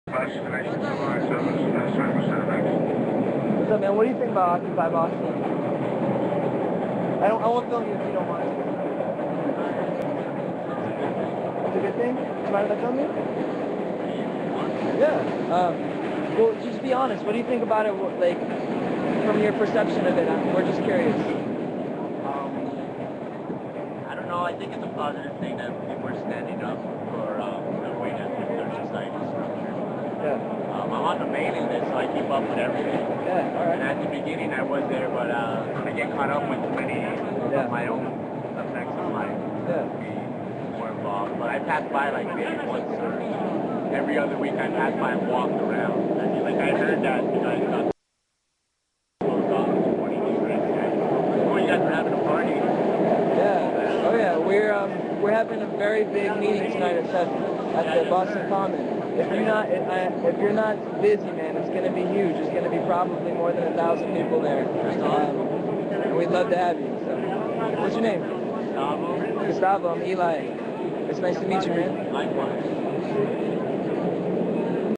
Interview with Gustavo
December 10th. The morning after the eviction from Dewey Square. We interviewed some people on their thoughts about Occupy Boston. This is Gustavo.